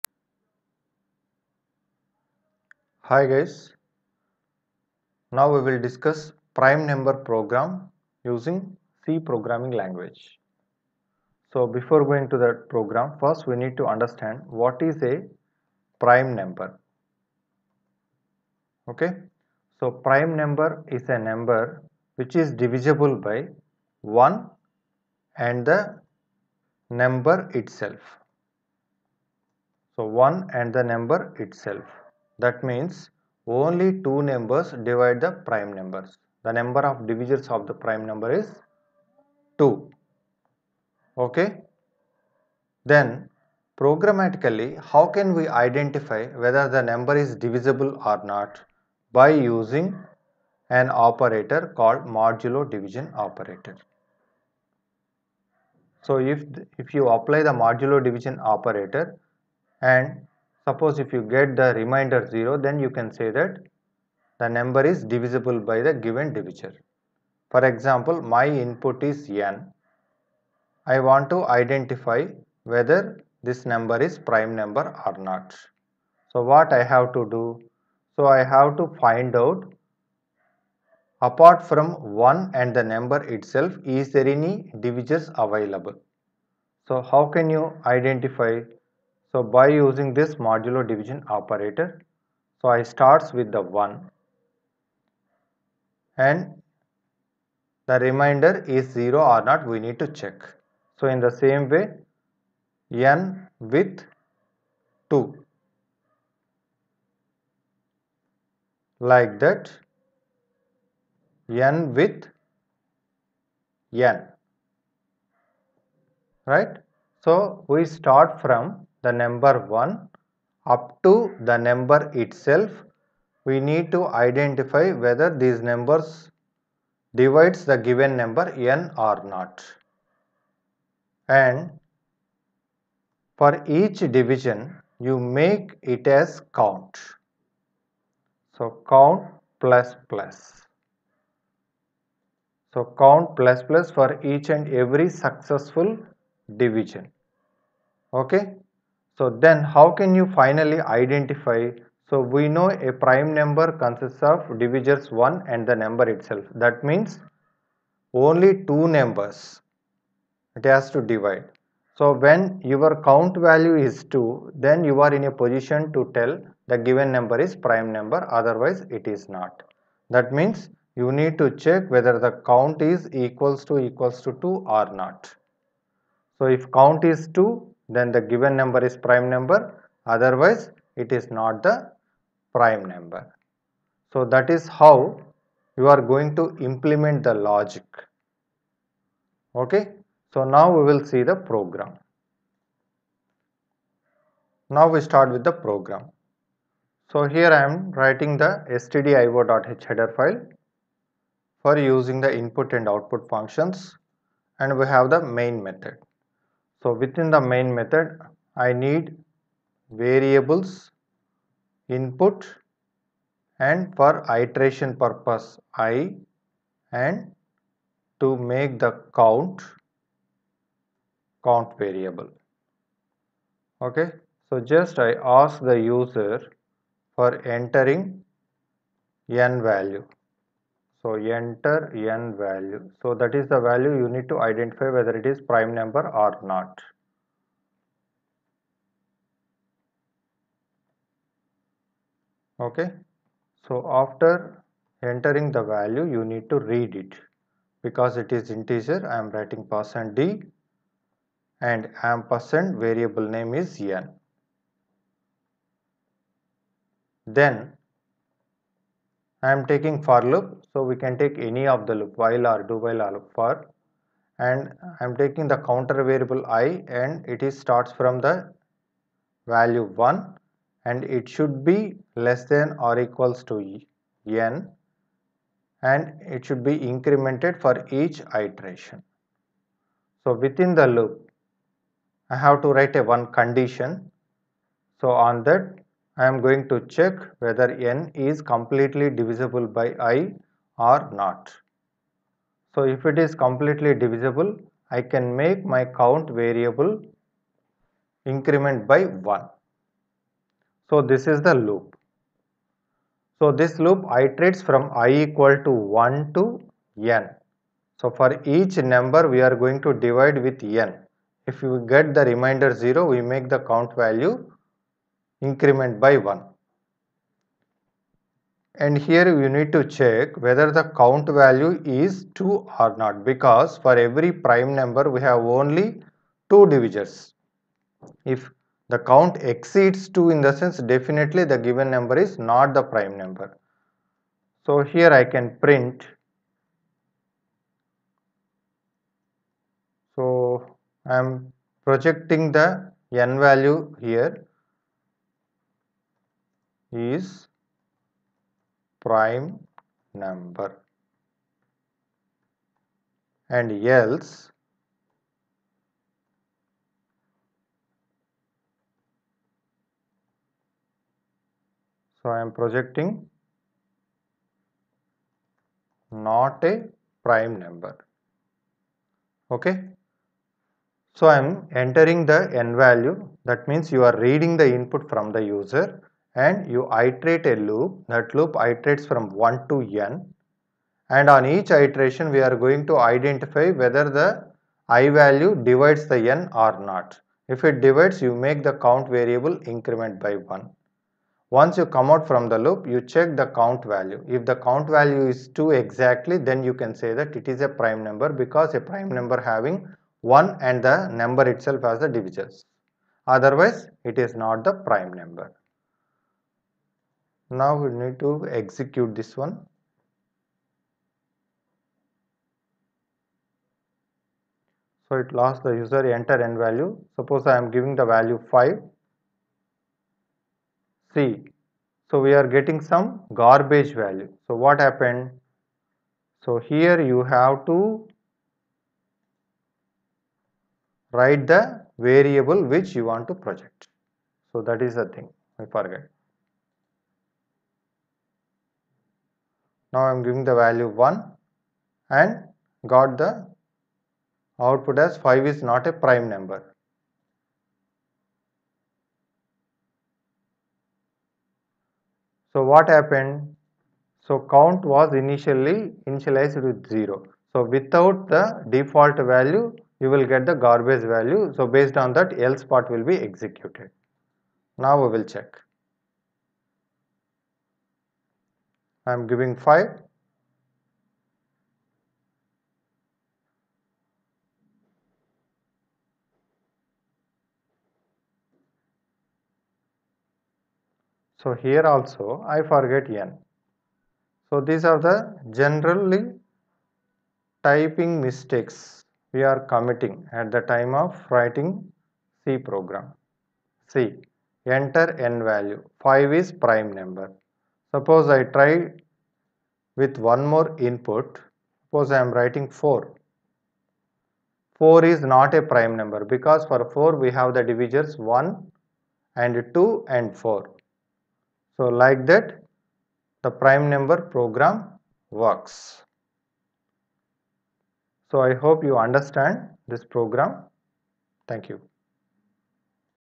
Prime Number Program in C Programming
Detail explanation of what is a prime number and explanation of how to write logic for identifying the prime number.